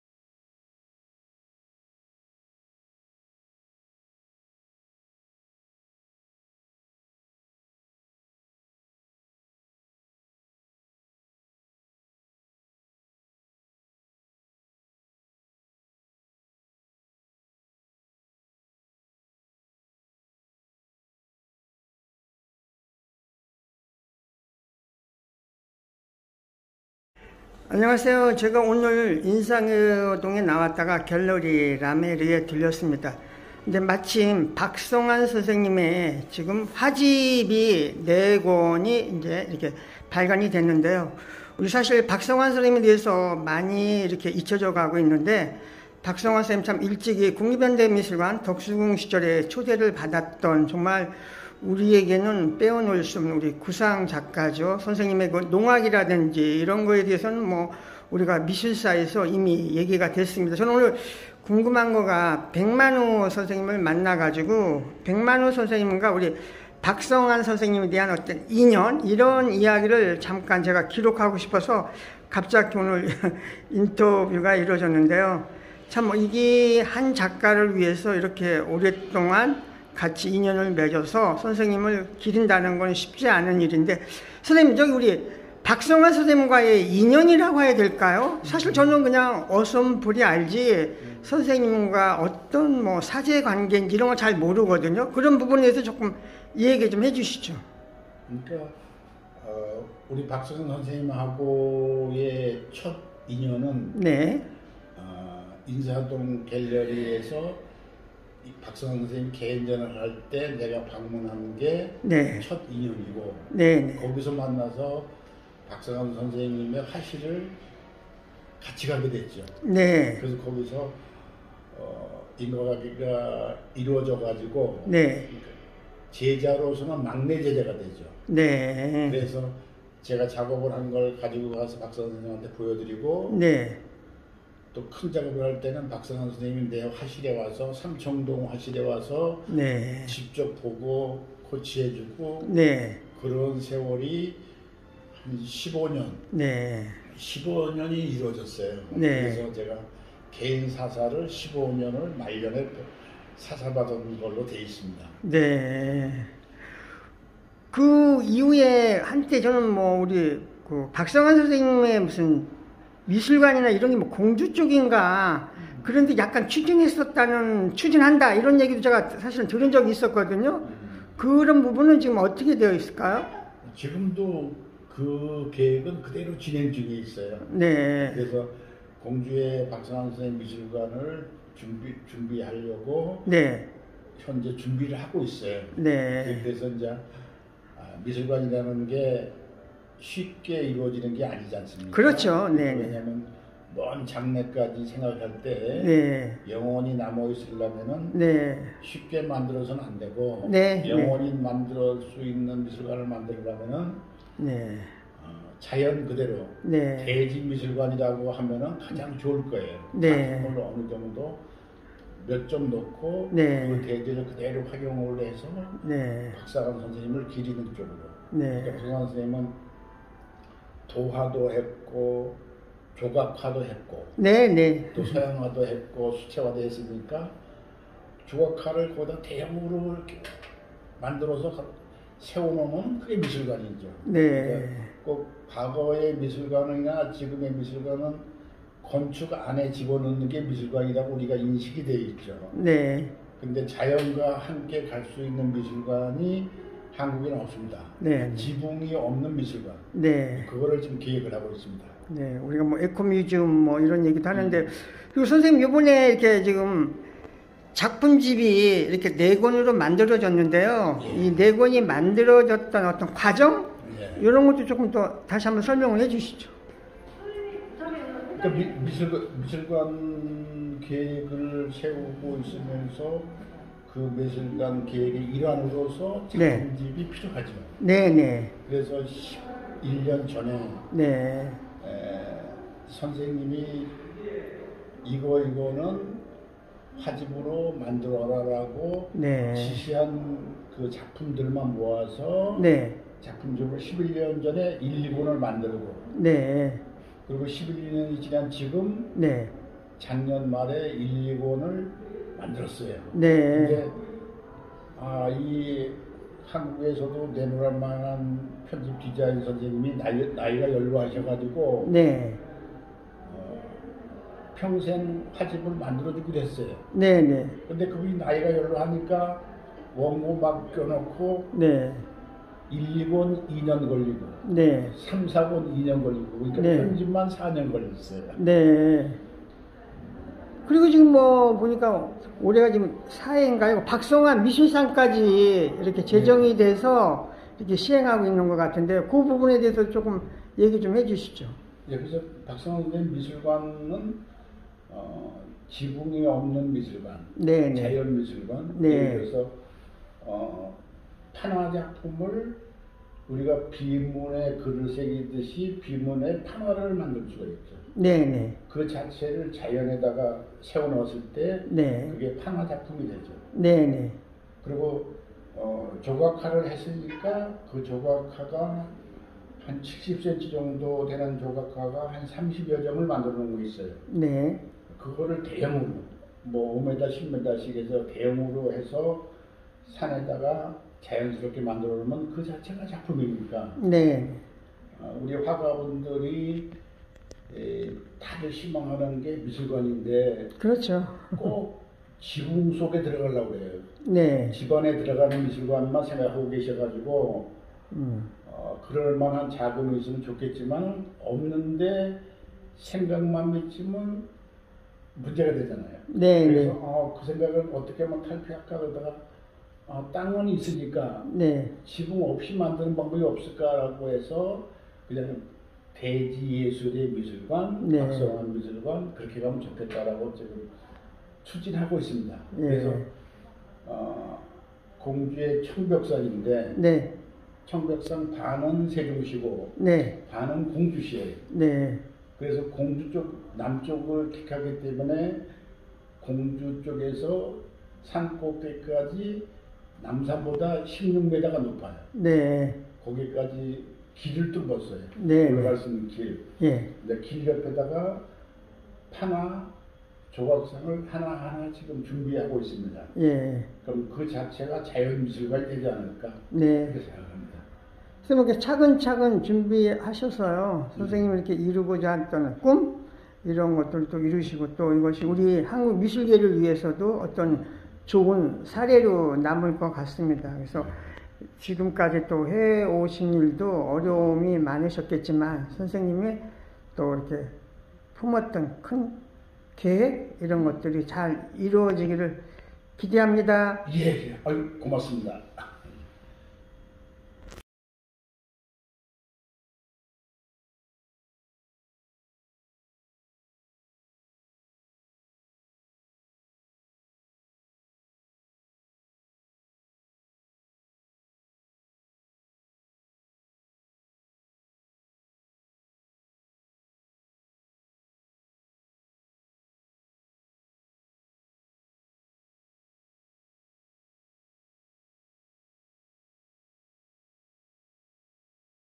(김달진미술사이야기) 박성환 농악 국제HAM예술제 백만우 인터뷰 2022.11.21. 갤러리라메르
박성환(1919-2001) 화집 4권이 발간되었다. 제자 백만우선생을 국제HAM에술제가 열리는 갤러리라메르에서 11.21. 인터뷰를 통해서 공주에 미술관 추진 이야기를 들었다.